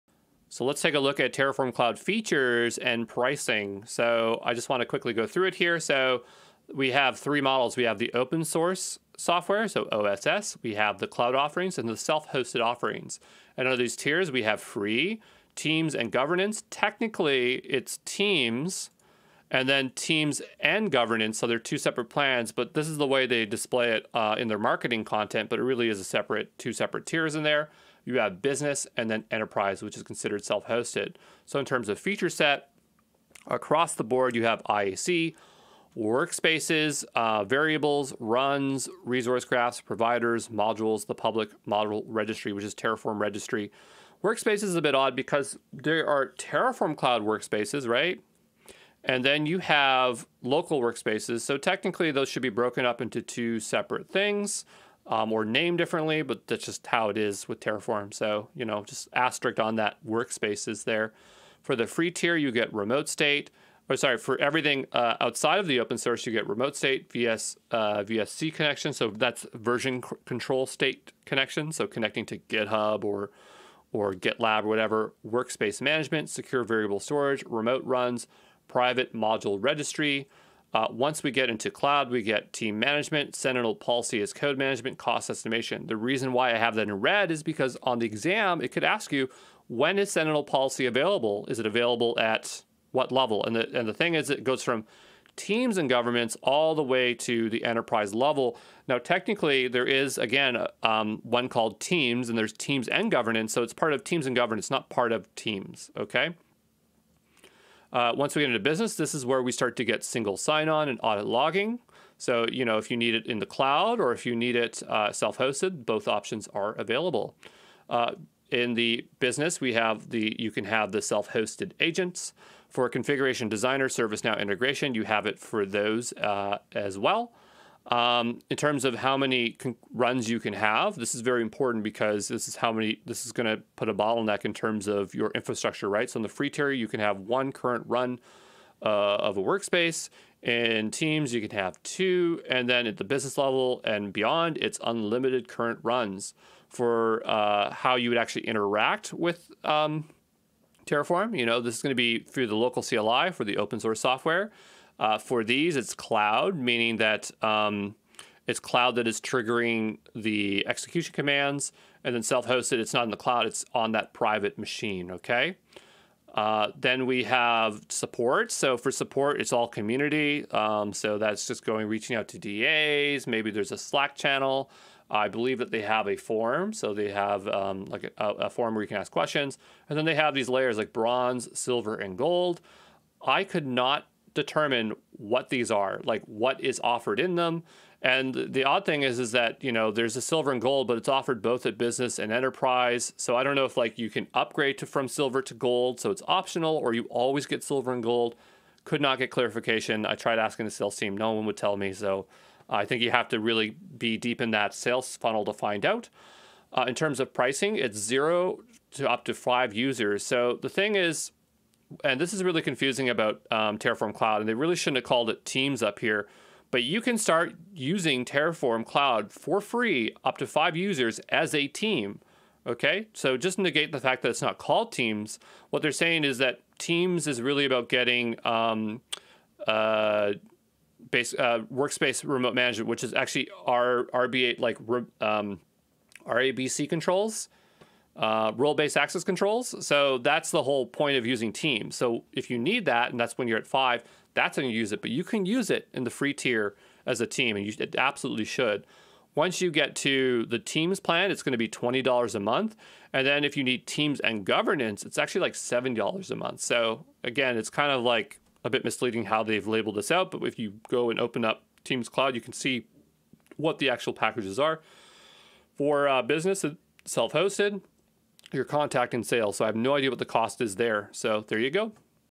Terraform Cloud Features and Pricing - HashiCorp Terraform Associate (003)
Get an overview of the features and pricing tiers available in Terraform Cloud. Learn how different plans cater to various organizational needs, from small teams to enterprise-level requirements.